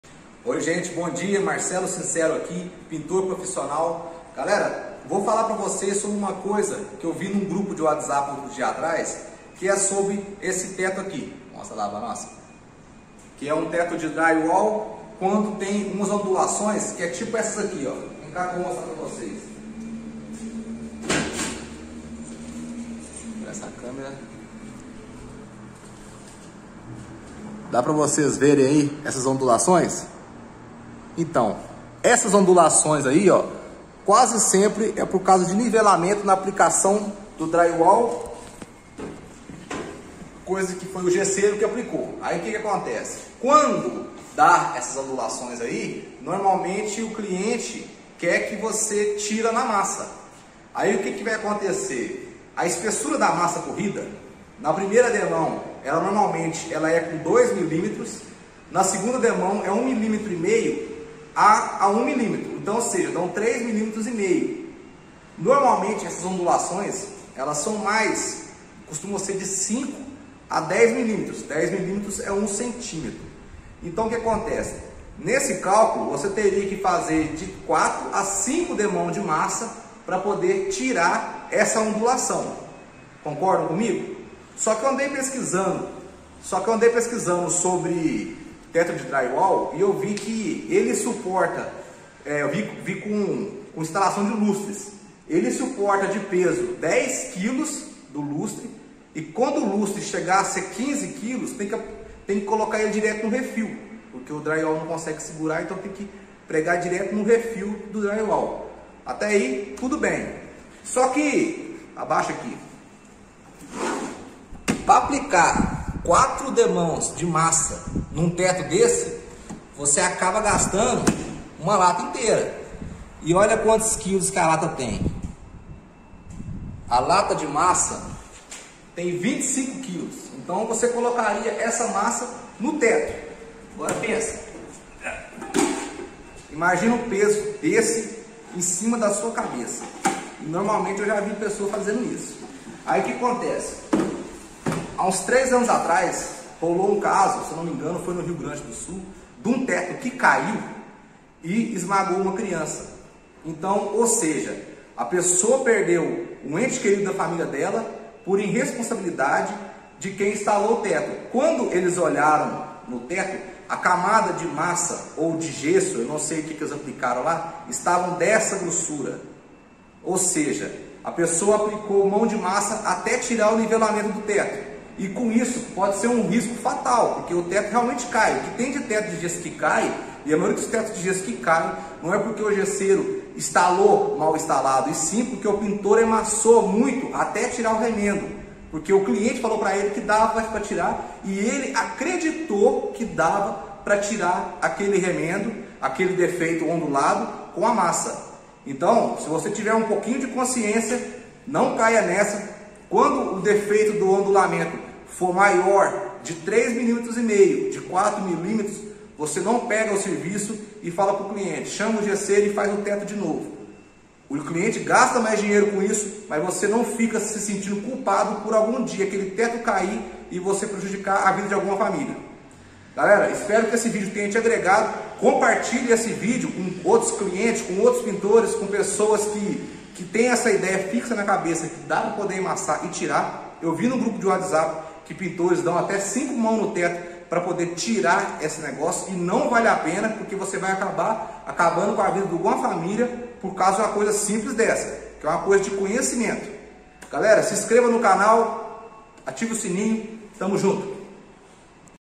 O problema do forro de gesso drywall ( CUIDADO!!!!)
Título:O Problema do forro de gesso drywall cuidado!
Nesse vídeo sobre o problema do forro de de gesso drywall eu falo sobre  algo simples, mas tão simples que  que pode acontecer na aplicação de gesso drywall no teto .
Se sua casa tiver rebaixamento de gesso no teto e ele estiver ondulado. TOME CUDADO

Title: Drywall plaster lining beware!
In this video I talk about something simple, but so simple that you shouldn't even make a video warning about it. But unfortunately little is known about this danger, so I felt I had a duty to make this ALERT!

Título: ¡Cuidado con el revestimiento de yeso para paneles de yeso!
En este video hablo de algo simple, pero tan simple que ni siquiera deberías hacer un video de advertencia al respecto. Pero, lamentablemente, se sabe poco sobre este peligro, por lo que sentí que tenía el deber de hacer esta ALERTA.